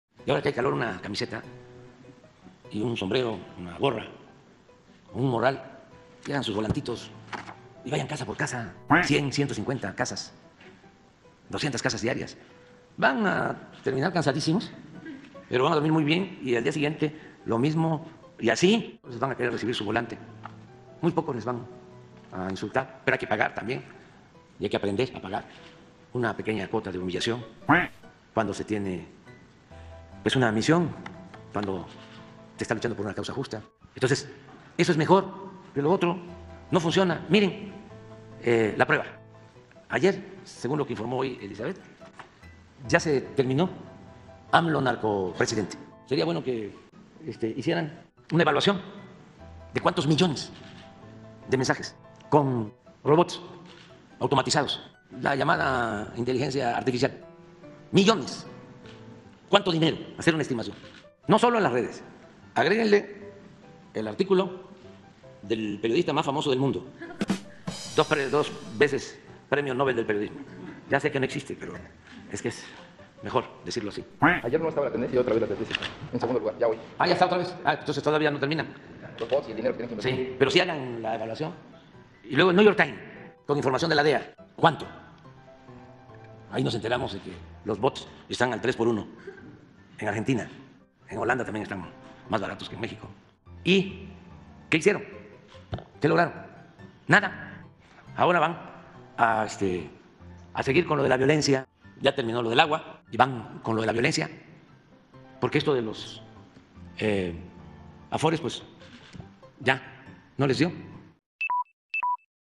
Recomienda a sus adversarios ir casa por casa, en lugar de hacer propaganda contra él en redes 🦆
Recomienda que sus adversarios vayan casa por casa a entregar volantes, en lugar de hacer propaganda contra él con robots automatizados 🦆, o con artículos del NYT porque “no logran nada” 🦆